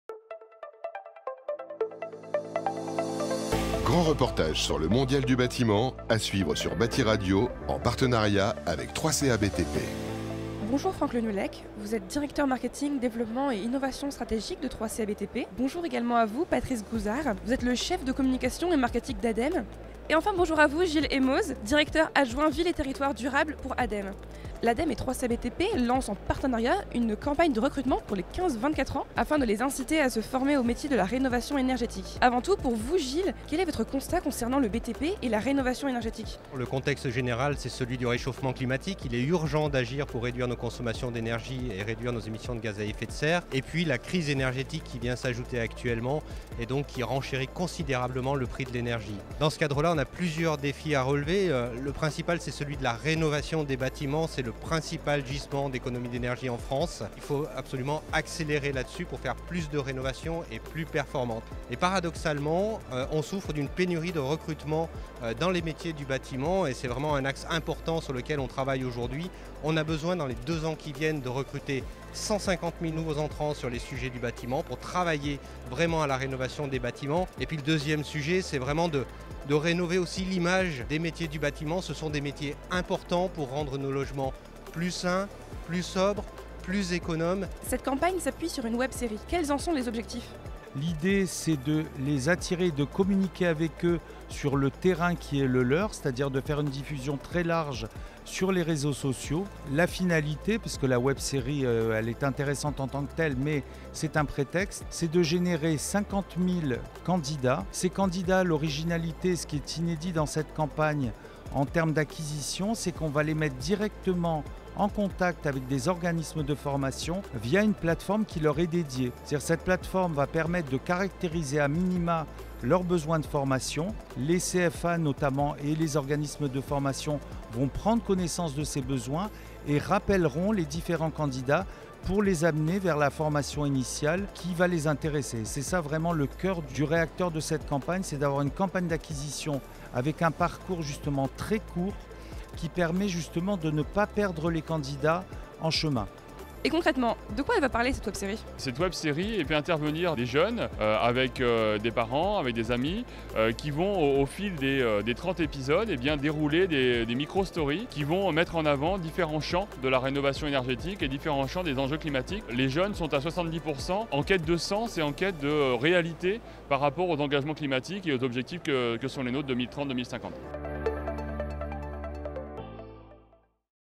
Lancement de la Websérie sur les métiers du BTP liés la rénovation énergétique - CCCA-BTP - ADEME
Pourquoi une webserie sur les métiers du BTP liés la rénovation énergétique ? De quoi parle cette série ? Quels sont les objectifs de l'Ademe ? et ceux du CCCA-BTP ?

Avec Franck Le Nuellec, Patrice Grouzard et Gilles Aymoz.